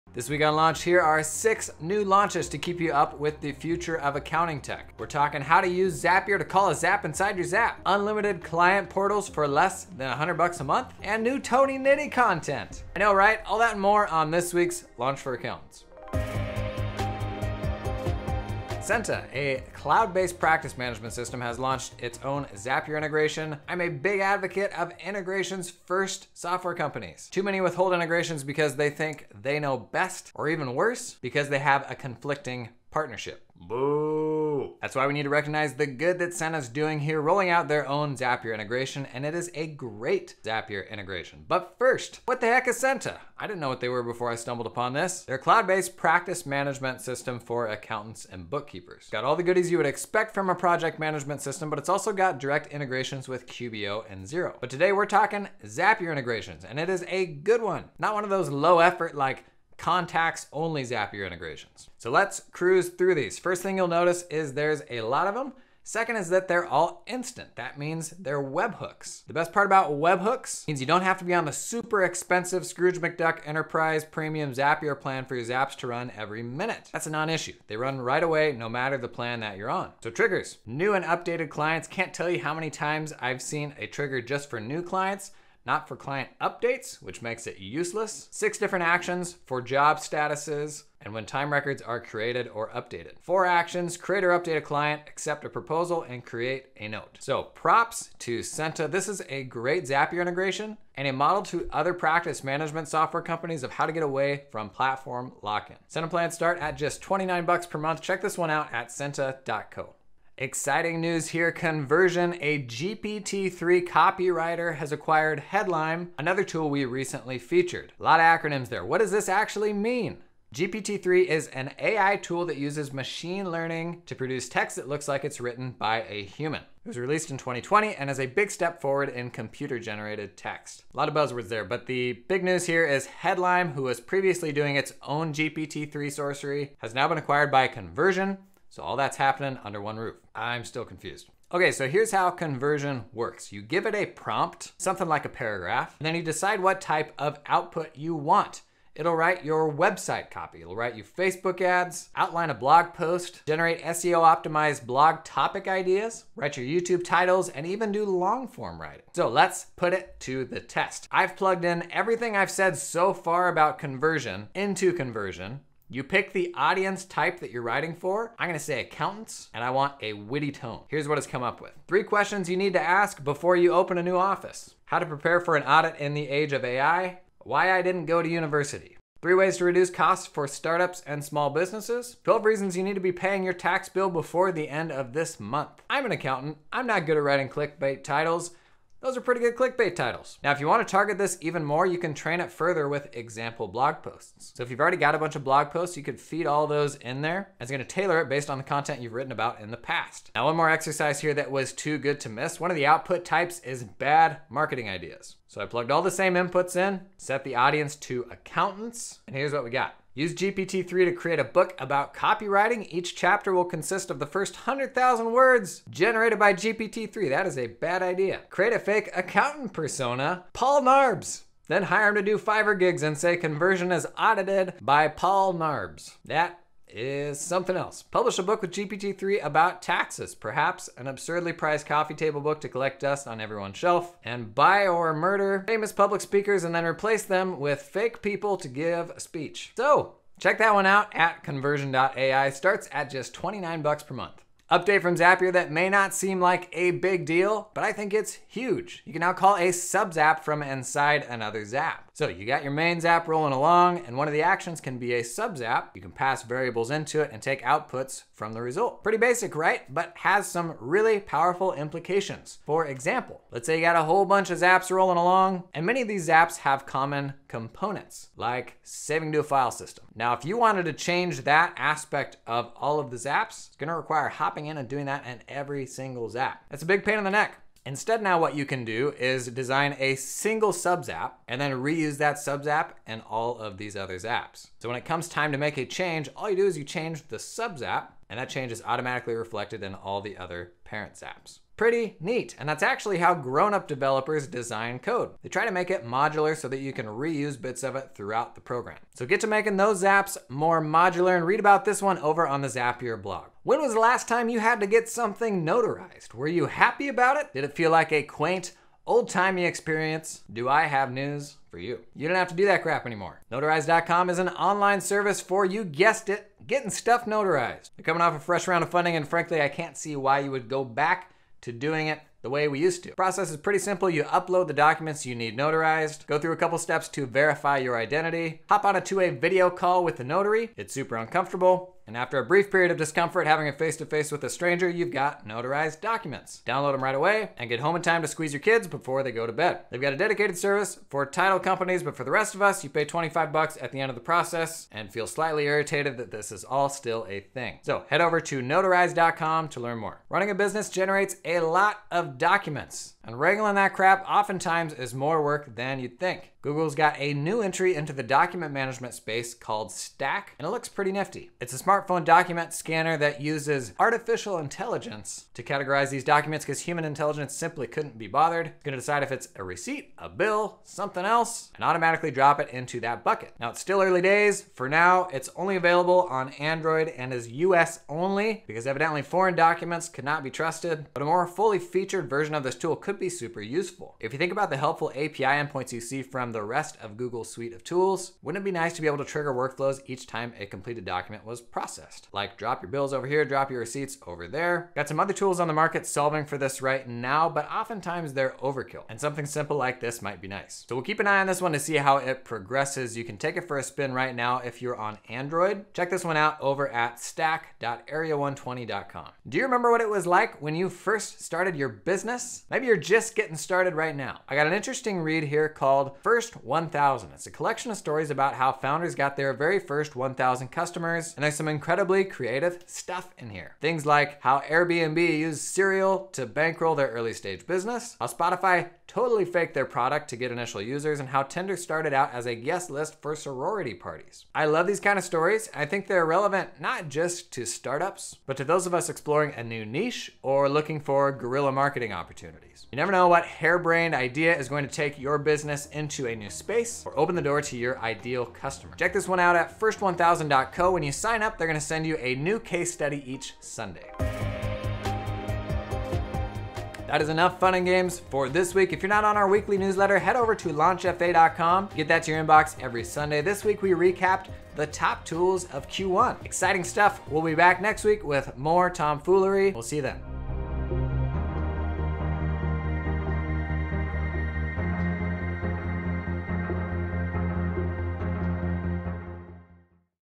How to call Zaps inside your Zaps to simplify your Zaps
*So I bungled the intro here, ignore the Portal + Nitti bits, apologies XD - Check out last week's video for those

0:00 Intro
0:18 Senta Zapier Integration
1:50 Conversion
4:04 Zapier Sub-Zaps
5:03 Notarize
5:50 Stack
6:49 First 1000

Not getting Launch in your inbox every week?
Curious how launches are included?
Are you a software company looking to connect with the accounting industry?